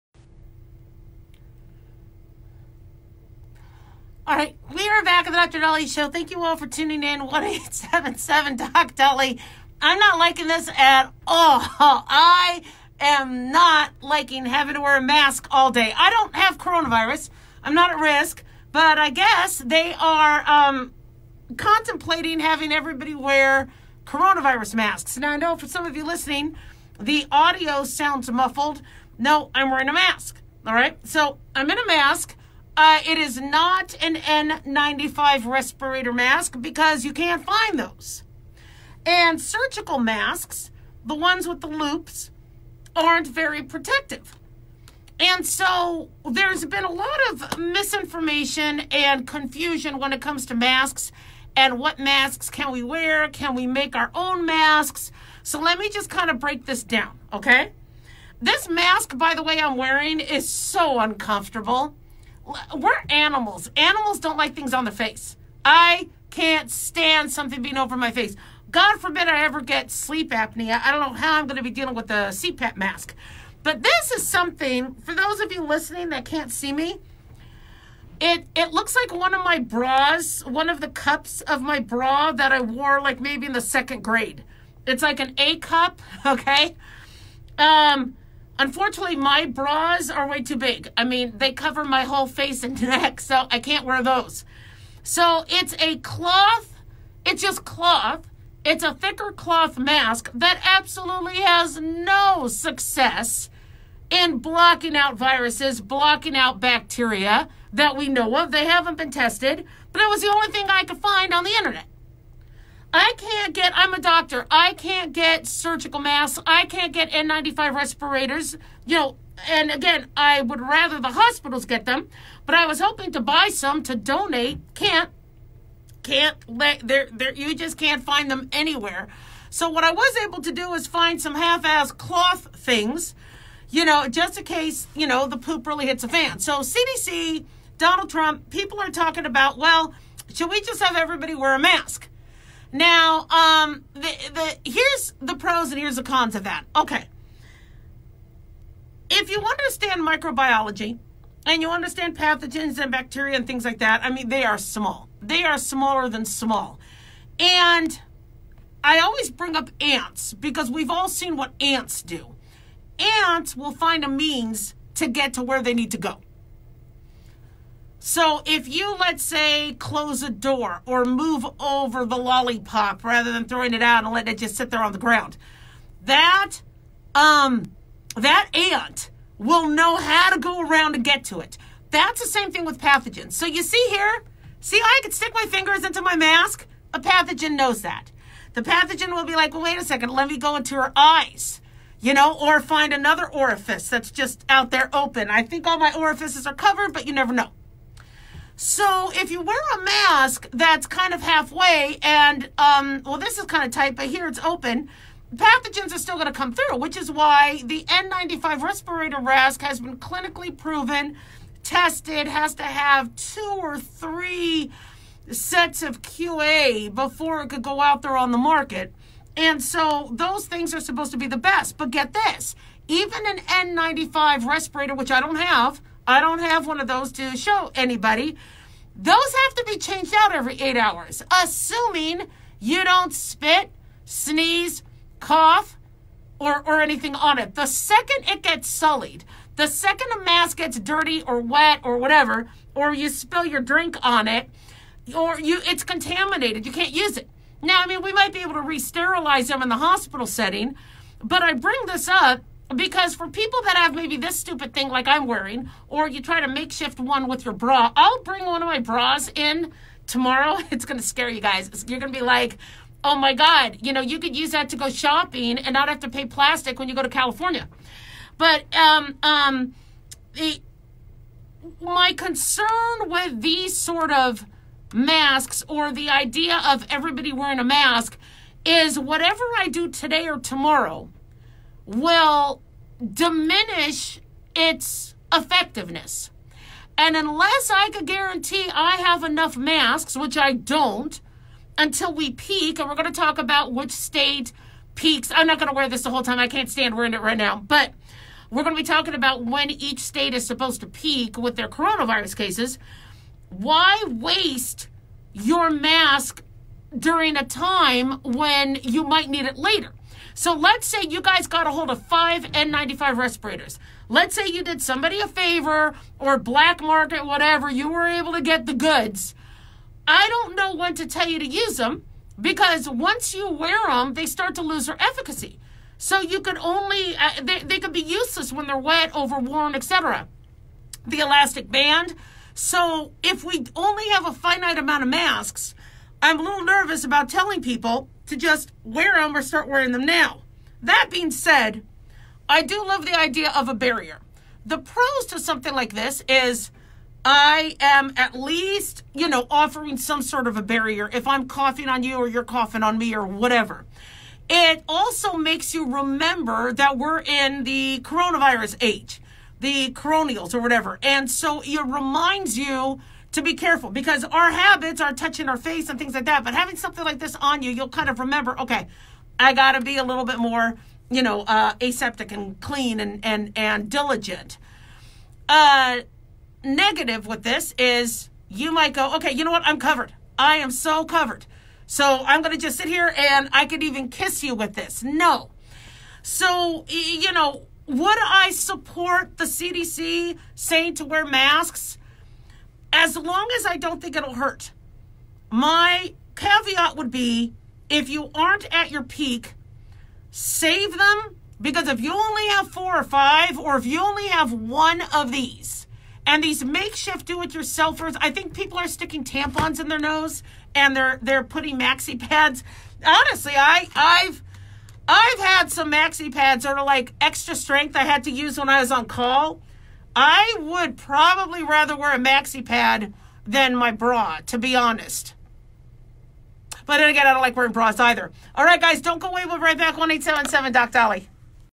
Coronavirus: Will Washington make us wear masks?
Dr. Daliah explains the pros and cons to wearing masks in an attempt to slow COVID spread.

Please note the mask is not an N95 respirator nor a mask that has been proven to protect one against pathogens.